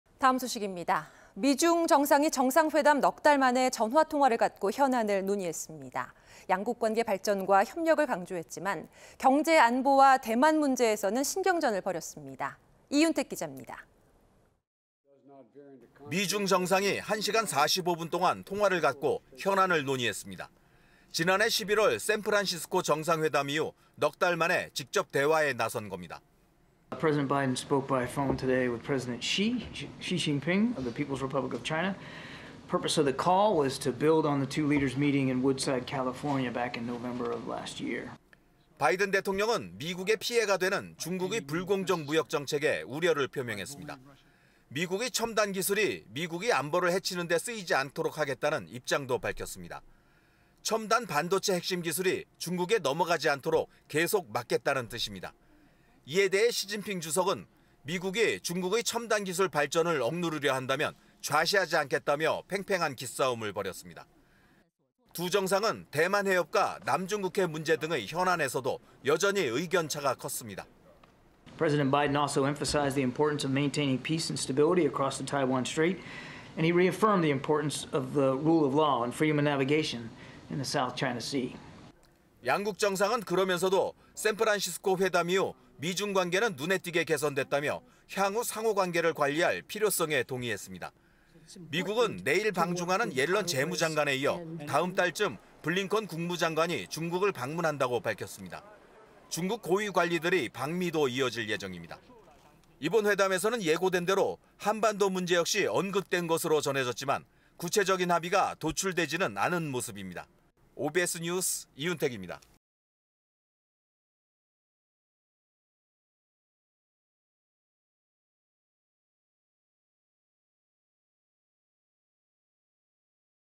4달 만에 통화한 바이든·시진핑, 경제안보·대만 문제 이야기하자..
【앵커】
미중 정상이 정상회담
넉 달 만에 전화 통화를
갖고 현안을 논의했습니다.
양국 관계 발전과 협력을
강조했지만, 경제안보와 대만
문제에서는 신경전을 벌였습니다.
이윤택 기자입니다.

【기자】

미중 정상이 1시간 45분동안 통화를 갖고 현안을 논의 했습니다.

지난해 11월 샌프란시스코 정상회담 이후 넉달만에 직접 대화에 나선 겁니다.

[존 커비/미 국가안보회의 대변인:바이든 대통령은 시진핑 주석과 통화했습니다. 목적은 지난 해 11월 캘리포니아에서 열린 두 정상의 회담을 기반으로 하기 위함이었습니다.0014-0026]

바이든 대통령은 미국에 피해가 되는 중국의 불공정 무역 정책에 우려를 표명했습니다.

미국의 첨단 기술이 미국의 안보를 해치는 데 쓰이지 않도록 하겠다는 입장도 밝혔습니다.

첨단 반도체 핵심 기술이  중국에 넘어가지 않도록 계속 막겠다는 뜻입니다.

이에 대해 시진핑 주석은 미국이 중국의 첨단 기술 발전을 억누르려 한다면 좌시하지 않겠다며 팽팽한 기싸움을 벌였습니다.

두 정상은 대만 해협과 남중국해 문제 등의 현안에서도 여전히 의견 차가 컸습니다.

[존 커비/ 국가안보회의 대변인:
바이든 대통령은 대만해협의 평화와  안정을
유지하는 게 중요하고, 남중국해에서 법치와
항해 자유가 중요하다는 걸 재확인했습니다.]

양국 정상은 그러면서도 샌프란시스코 회담 이후 미중 관계는 눈에 띄게 개선됐다며 향후 상호 관계를 관리할 필요성에 동의했습니다.

미국은 내일 방중하는 옐런 재무장관에 이어 다음 달쯤 블링컨 국무장관이 중국을 방문한다고  밝혔습니다.

중국 고위 관리들의 방미도 이어질 예정입니다.

이번 회담에서는 예고된대로 한반도 문제 역시 언급된 것으로 전해졌지만, 구체적인 합의가 도출되지는 않은 모습입니다.

OBS뉴스 이윤택입니다.